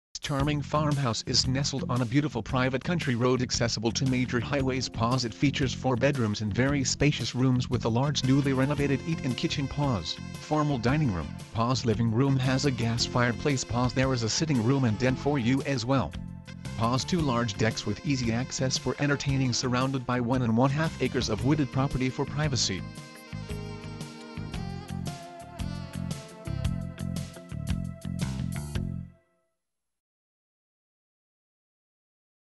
507 Hedden Rd Andover NJ
This charming farmhouse is nestled on a beautiful private country road accessable to major highways it features 4 bedrooms and very spacious rooms with a large newly renovated eat in kitchen, formal dining room, living room has a gas fireplace, there is a sitting room and den for you as well Two large decks with easy access for entertaining all surrounded by one and one half acres of wooded property for privacy

Video uploaded on Wed 23 Jun, 2010
real estate Andover NJ